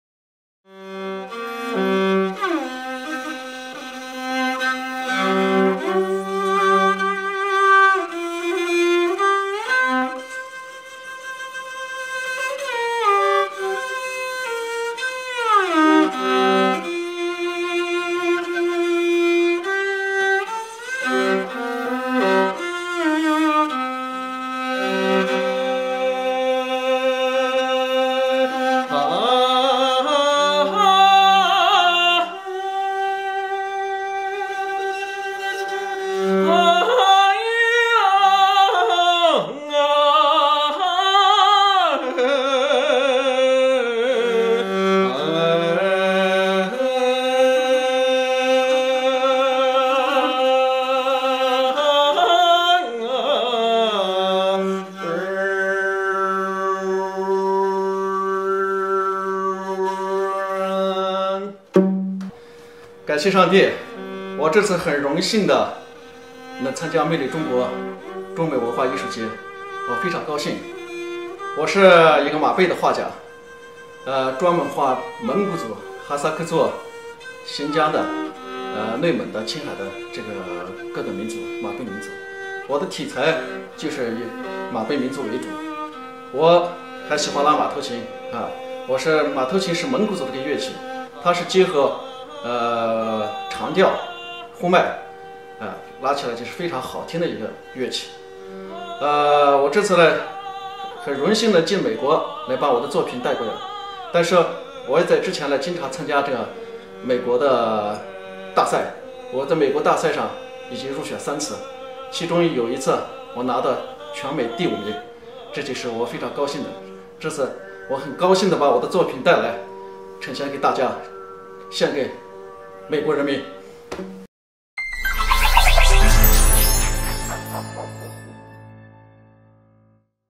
龙鹰电视: 画家裴发邦参加 2017魅力中国-中美文化艺术节
龙鹰电视:  画家裴发邦参加 魅力中国-中美文化艺术

2017 Magnificent China - US-China Cultural Art Festival